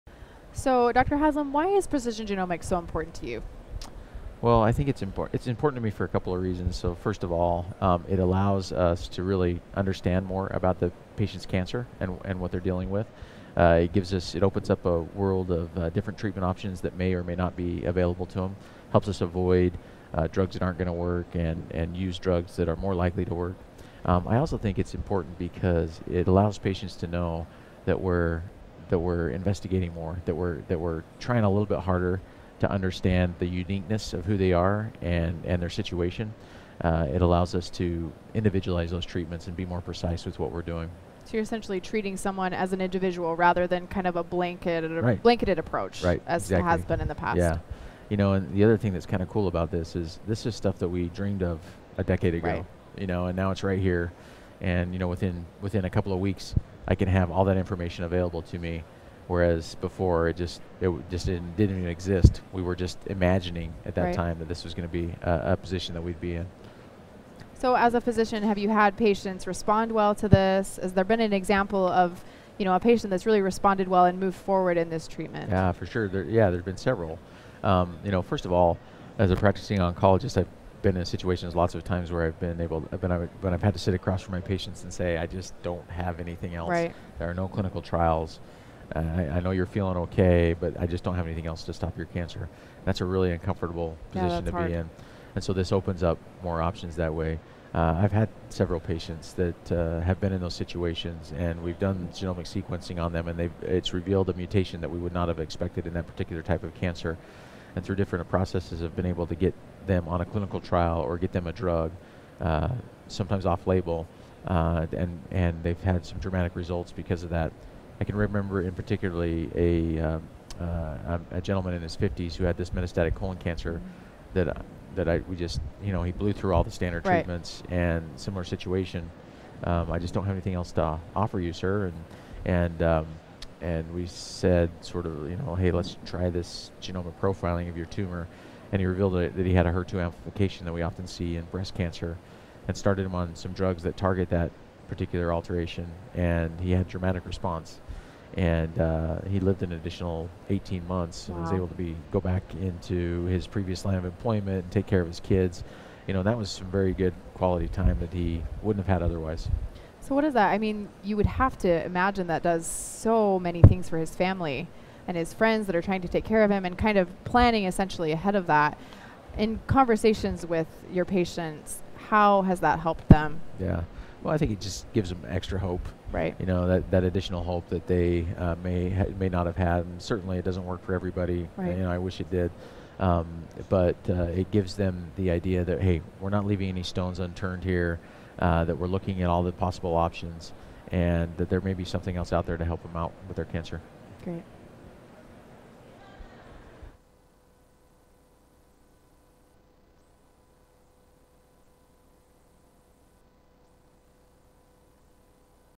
Why is Precision Genomics Important to You?
Dr. Derrick Haslem, Director of Medical Oncology at Intermountain Precision Genomics shares why precision genomics is important to him.